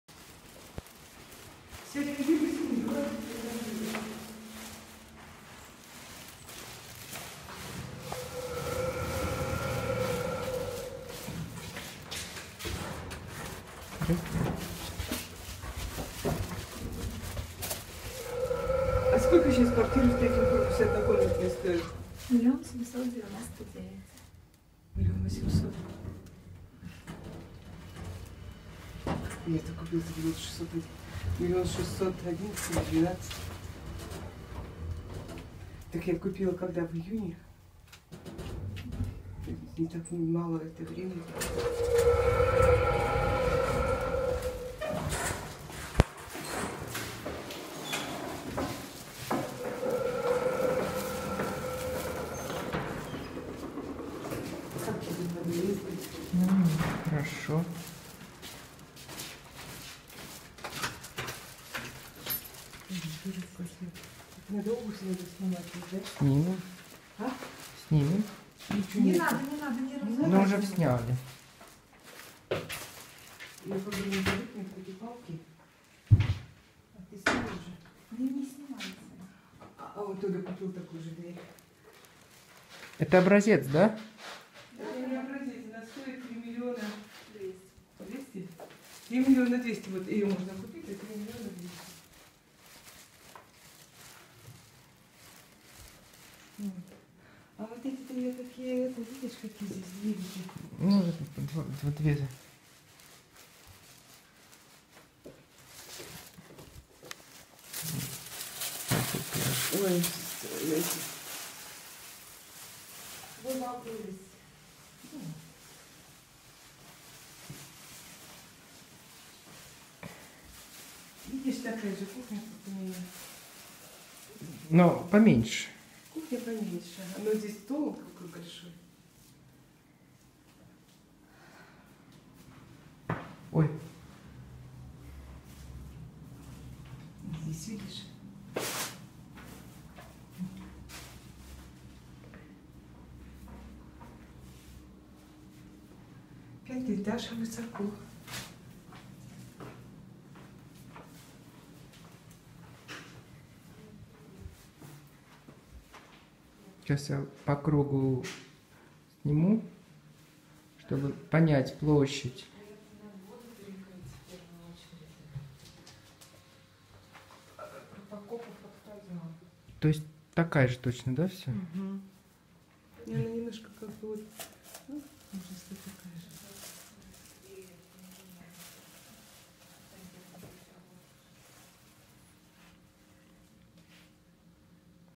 1 bedroom Flat for Sale in Ryazan, Grand Comfort Properties
My mom wants to sell her 2 bedroom flat and buy 1 bedroom flat in this housing association.